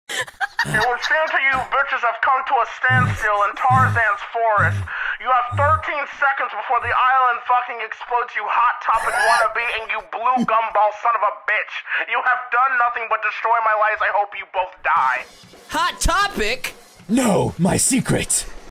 standstill in Tarzan's forest - X-RL7 animatic
an alternative turn of events at Xeno's gathering